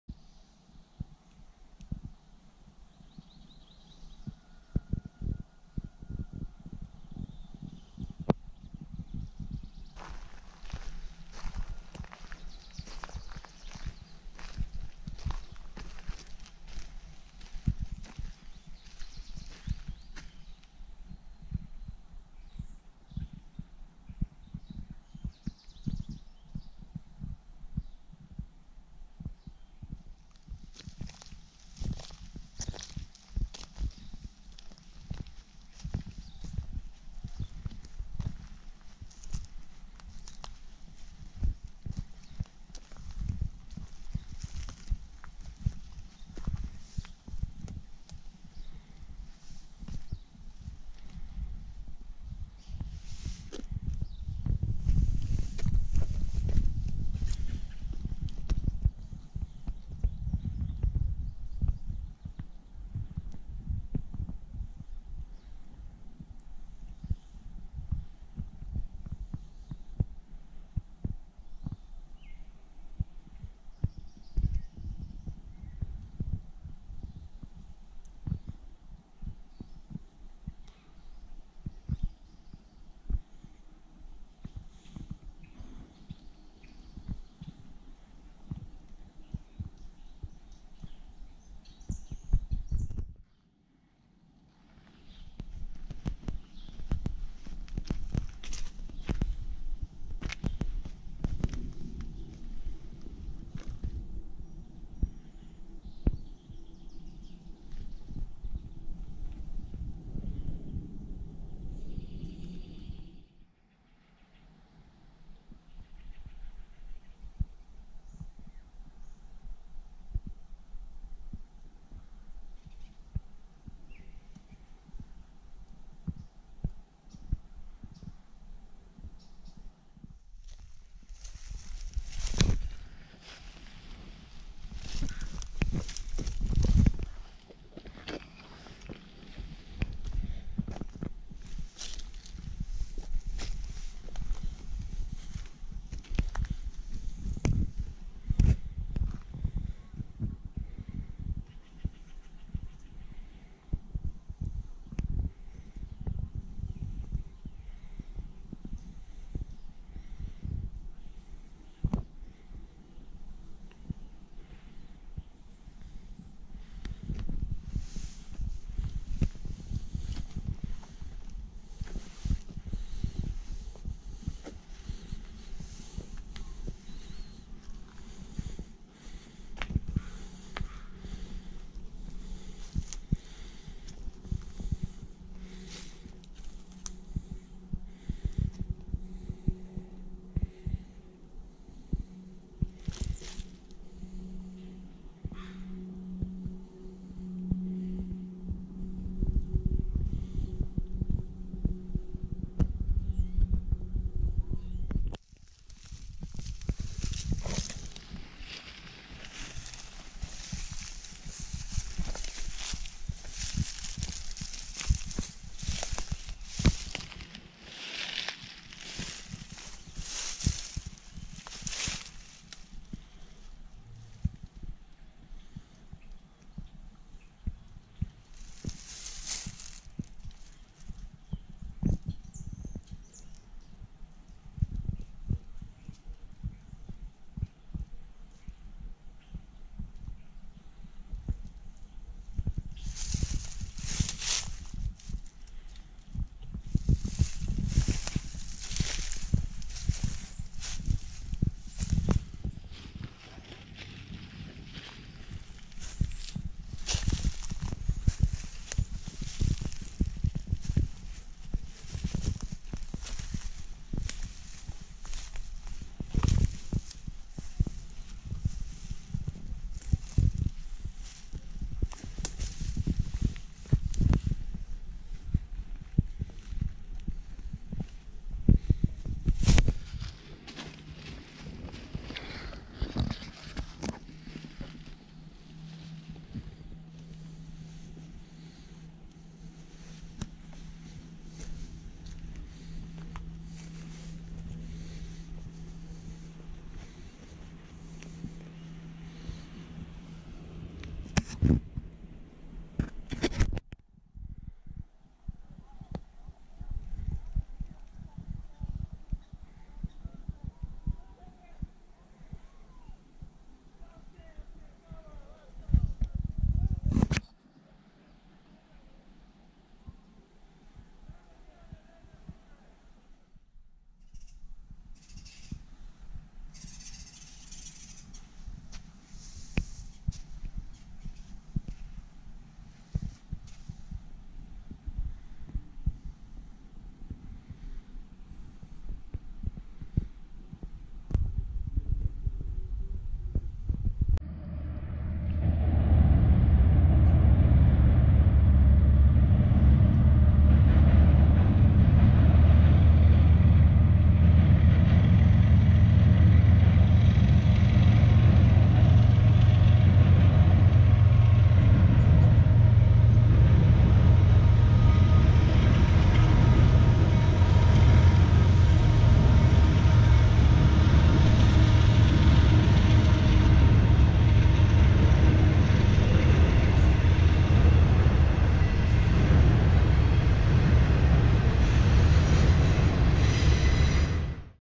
Exploring Abandoned Mill Site - Rat Portage, Harrison Mills.
Rat Portage Lumber Mill in Harrison Mills, B.C.. Not much stands today, but it is in a Beautiful Spot of B.C..  Come to check it out on a Hot Sunny Day, or Come in November to catch a Large, Beautiful Eagle Migration to this Area.

Best.  Beautiful British Columbia.